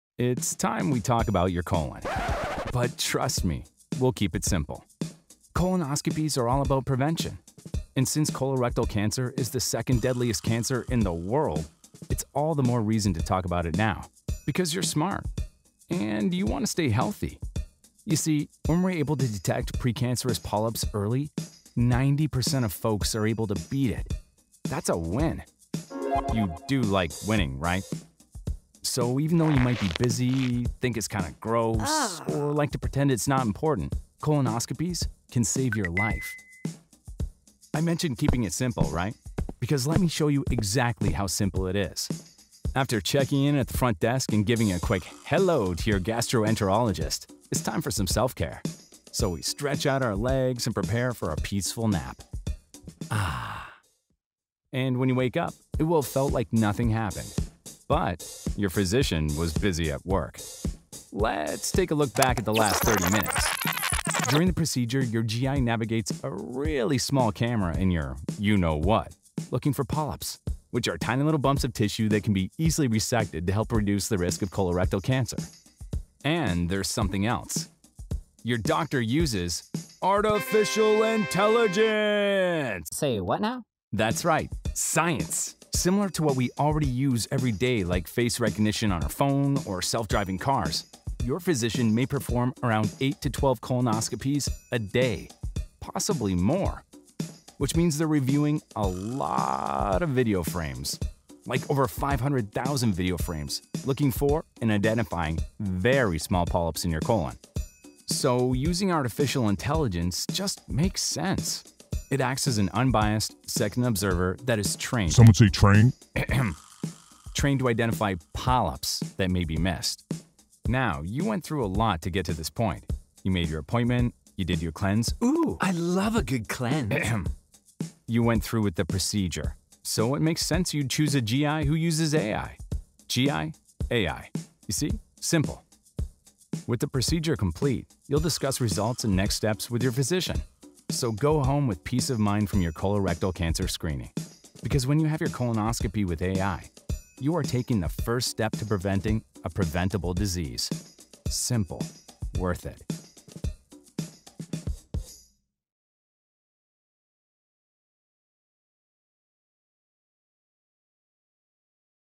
So your GI uses AI
This is a patient instructional video explaining how GI Geniusᵀᴹ Module works with gastroenterologists during colonoscopies.
(US-DG-2100281)